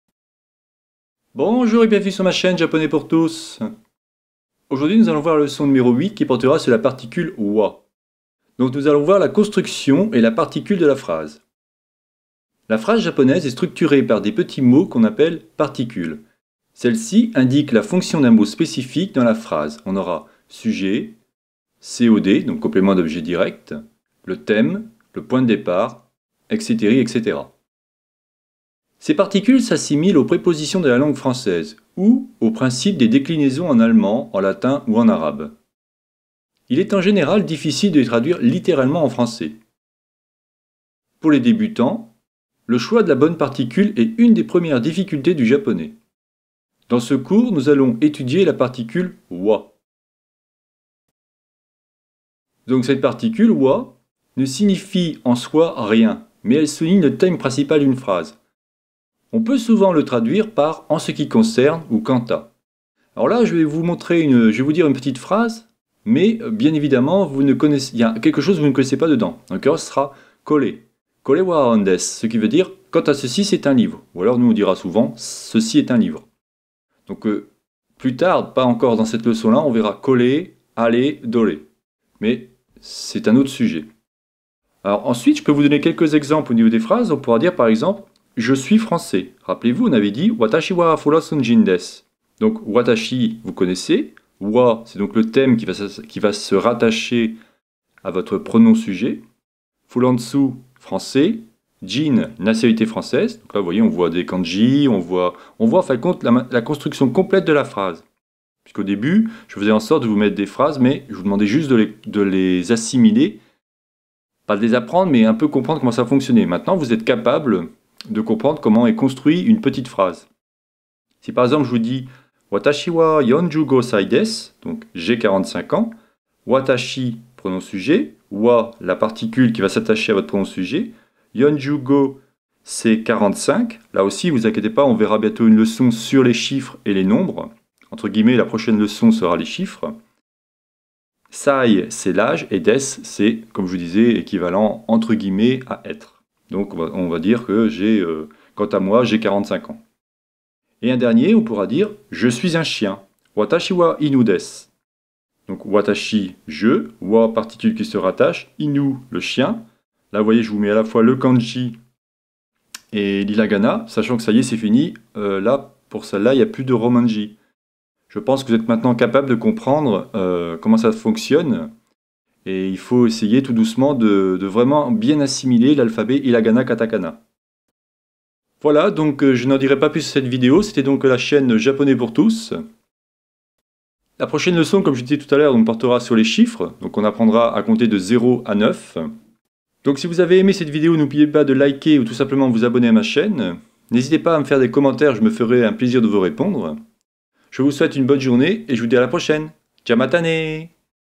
Japonais - 08 - Particule wa
Sur cette vidéo, nous allons voir ensemble comment fonctionne la particule wa et surtout quelle est son utilité.